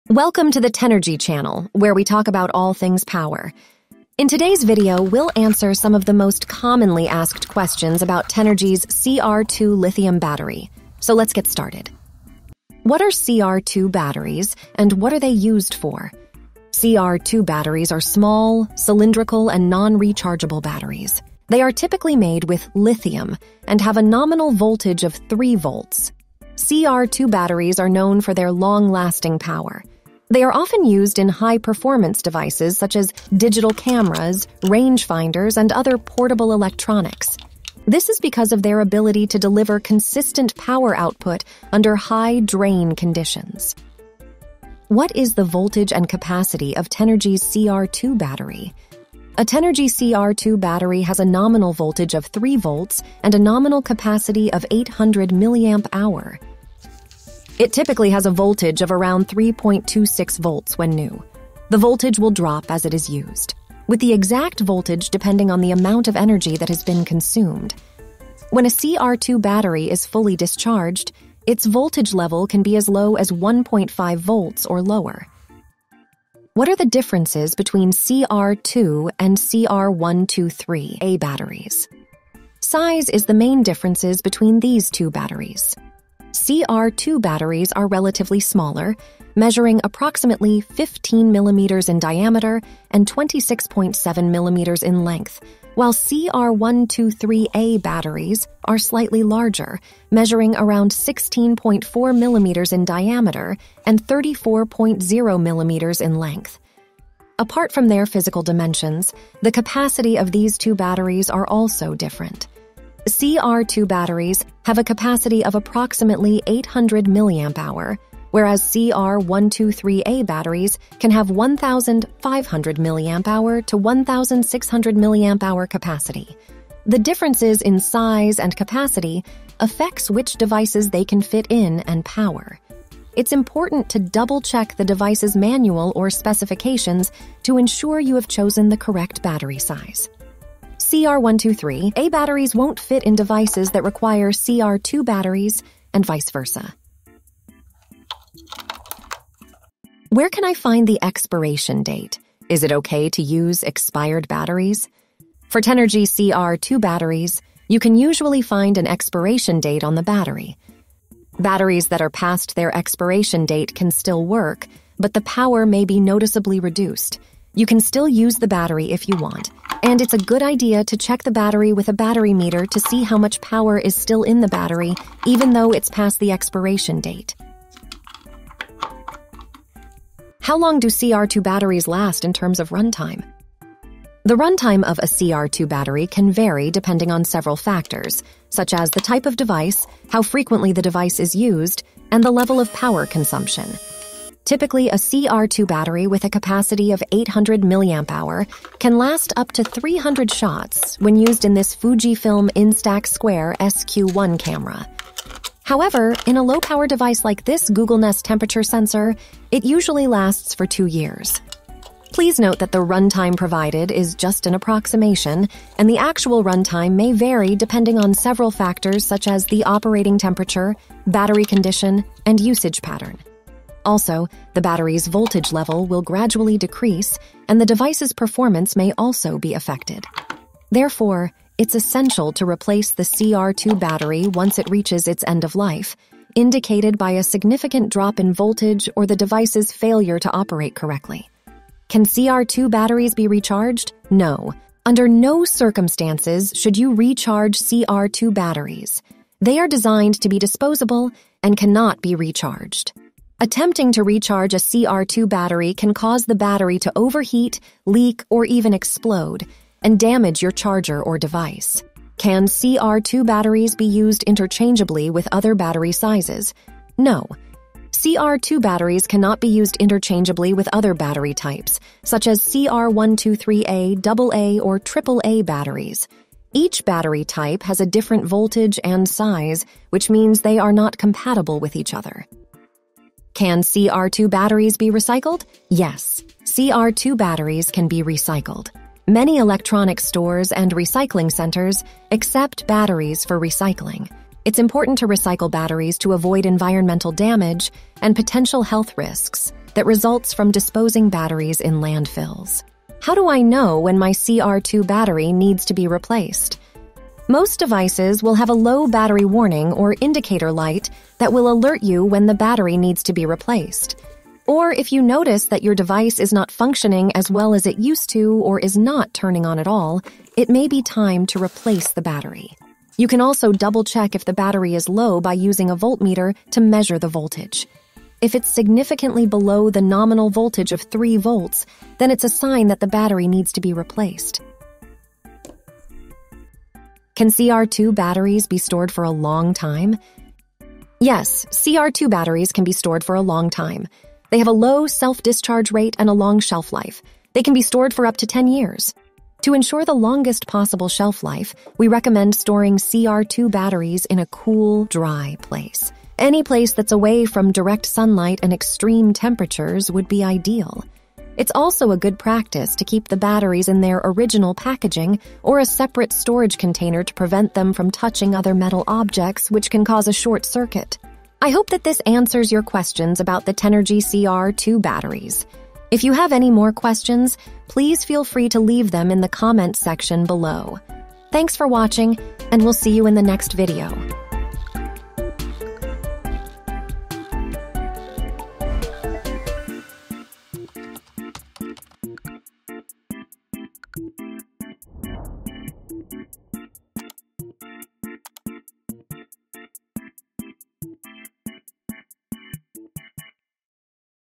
Answering frequently asked questions about Tenergy's CR2 batteries
In this Q&A video, we answer some of the most frequently asked questions about the Tenergy's CR2 batteries.

Looking for info we didn't cover? Drop us a question in the comment section, we're always around to help out.

Check out the Tenergy channel for our latest products, how-tos, unboxing, and more.

WHAT YOU’LL SEE:
0:00 Intro
0:12 What are CR2 batteries?
0:45 CR2 batteries voltage and capacity
1:16 Differences between CR2 and CR123A
2:30 Expiration date
3:00 Runtime
4:02 Can CR2 batteries be recharged?
4:22 Interchangeably other battery sizes?
4:45 Recycle
5:03 When to replace CR2 batteries
5:37 Storage
6:24 Outro